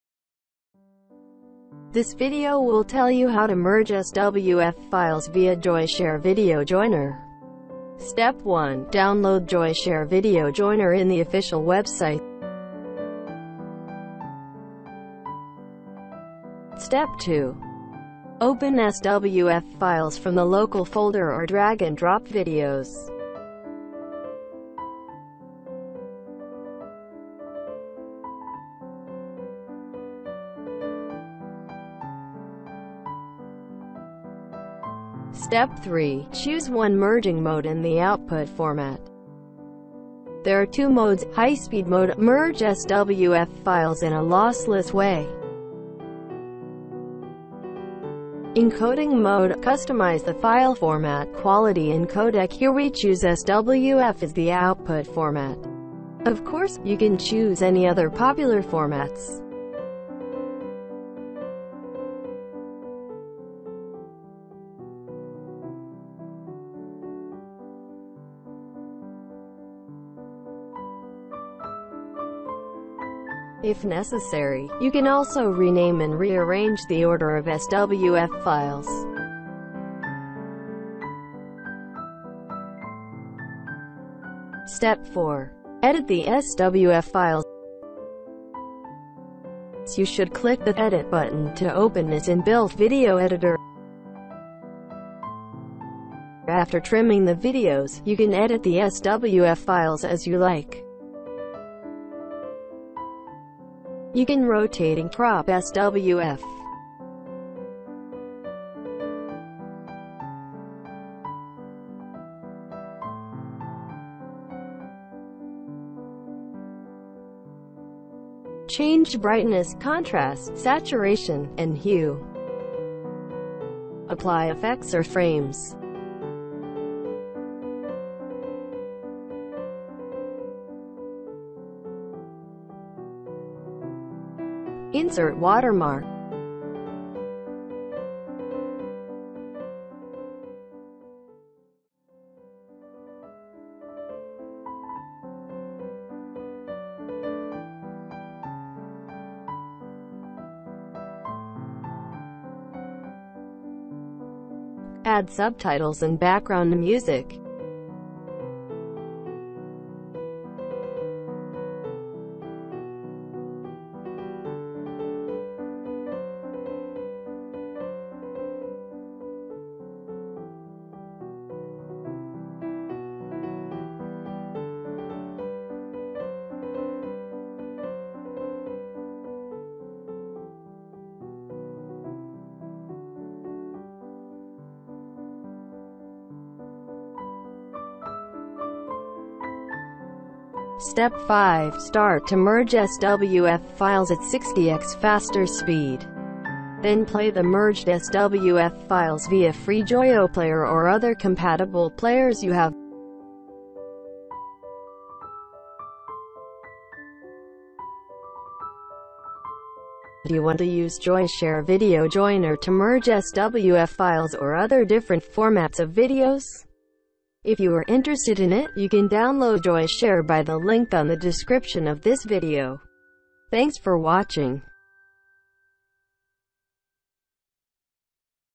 [Solved] How to Merge SWF Files into One without Hassle
Are you looking for an easy way to combine SWF files into one? Keep watching and you won't be disappointed! This guide will show you how to merge SWF videos by Joyoshare Video Joiner. You just need to take several steps as follows:

0:00 Joyoshare Video Joiner - Combine SWF Videos
0:10 Step 1. Download and Install Joyoshare Video Joiner
0:20 Step 2. Run Joyoshare and Import SWF Files
0:35 Step 3. Select One Mode and the Output Format
1:24 Step 4. Edit SWF Videos
3:10 Step 5. Export Merged SWF Video